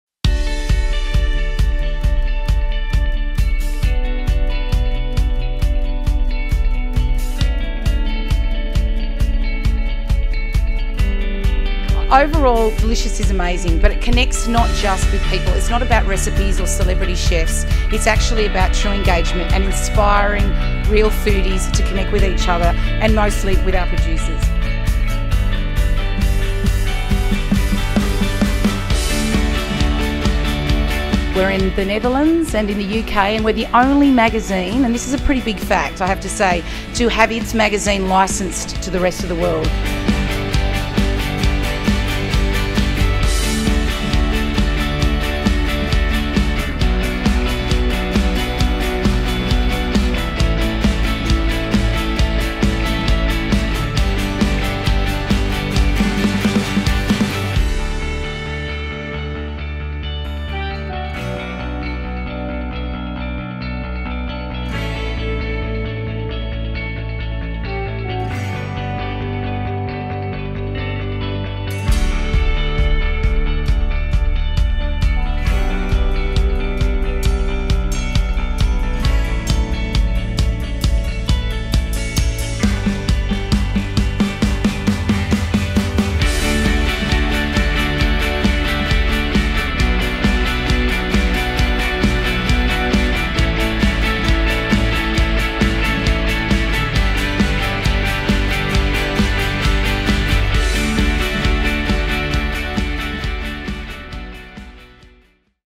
Produce Awards 2013 - Awards night
Come and relive the night as look back on the Produce Awards from 2013.